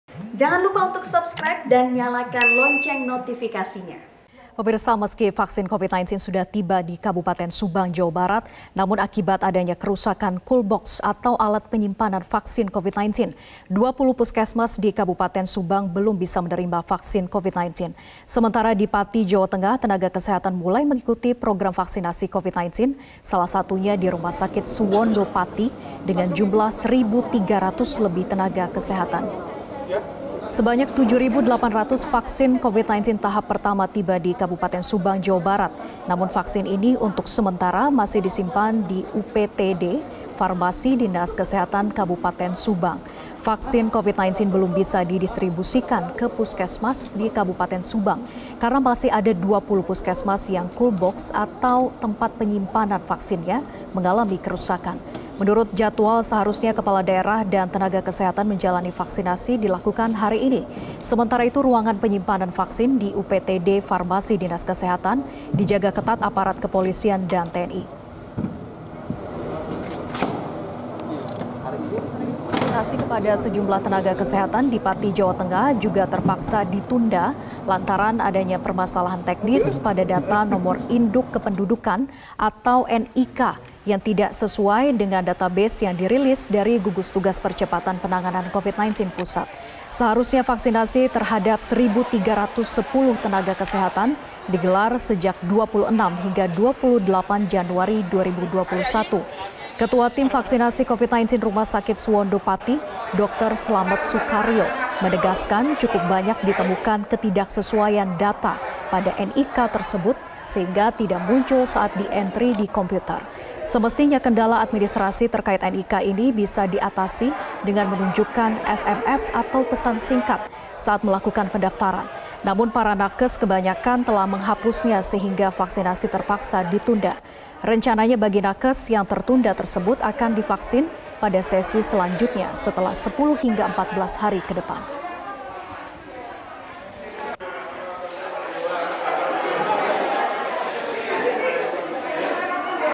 Vaksinasi Terkendala Masalah Teknis dan Cool Box Rusak
Sebanyak 7.800 vaksin covid-19 tahap pertama tiba di Kabupaten Subang, Jawa Barat. Namun vaksin belum bisa didistribusikan karena tempat penyimpanan vaksin atau cool box untuk 20 puskesmas rusak. Sementara di Pati, Jawa Tengah, vaksinasi tertunda akibat permasalahan teknis pada data NIK.